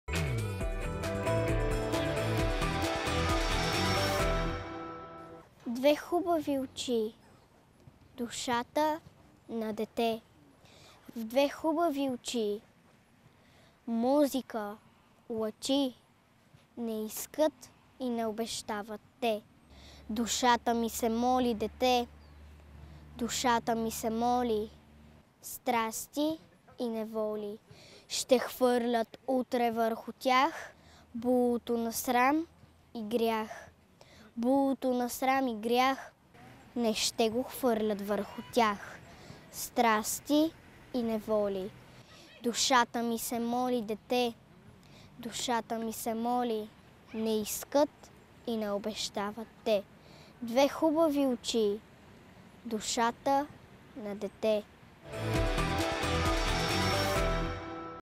„Родолюбие“ - "Две хубави очи" от Пейо Яворов
В рубриката „Родолюбие“ ви представяме стихотворението „Две хубави очи“ от Пейо Яворов. Рецитира Карина Николова